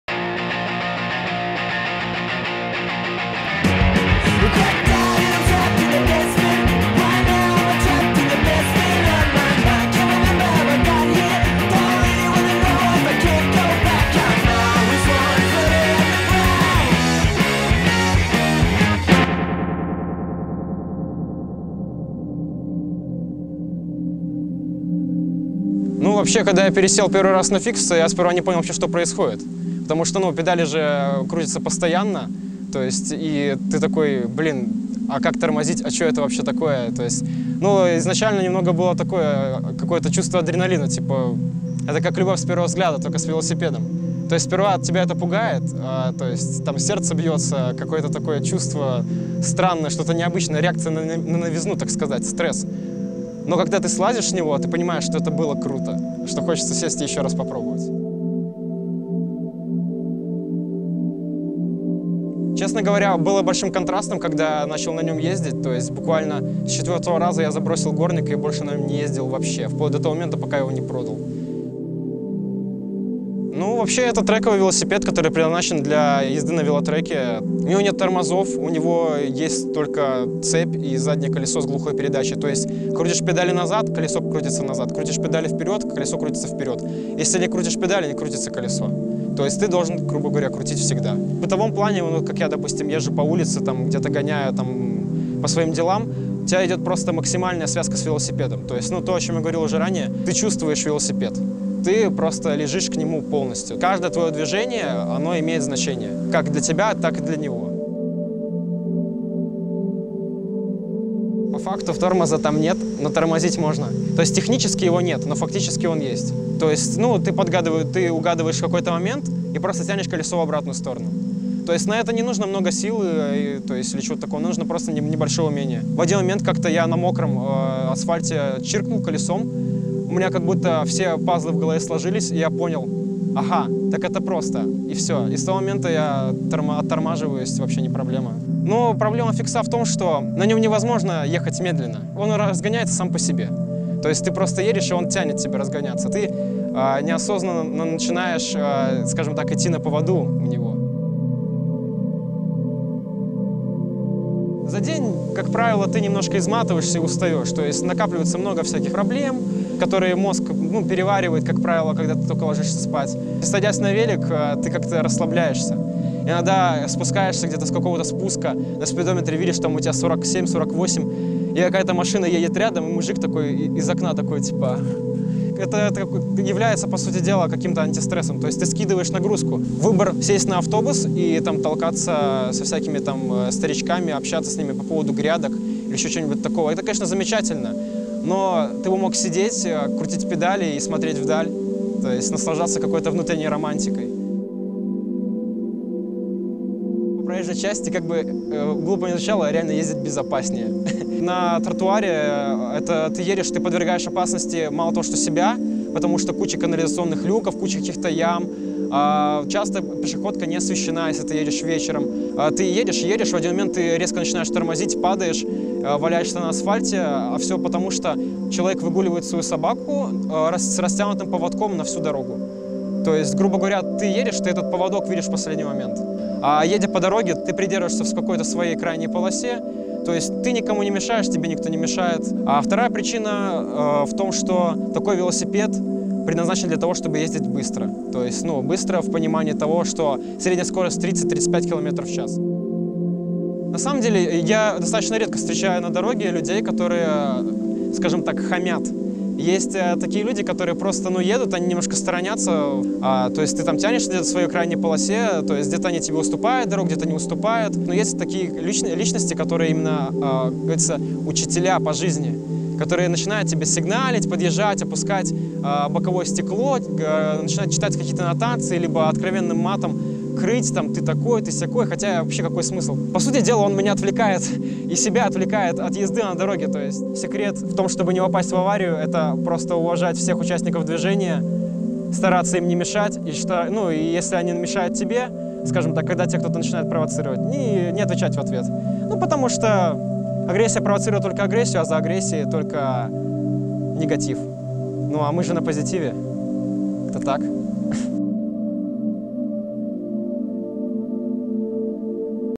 Одержимость: Парень без тормозов - история минского фиксера
У велосипеда с фиксированной передачей нет свободного хода: крутится колесо — крутятся педали. Также у него нет тормозов, зато есть задний ход — нужно крутить педали в обратную сторону. Вообще, такие модели придумали для езды на треках, но есть фанаты и на улицах города. С одним из них мы встретились. Смотрите видеорепортаж, подготовленный в рамках совместного с life:) проекта «Одержимость».

Использование материалов Onliner.by возможно только с разрешения редакции.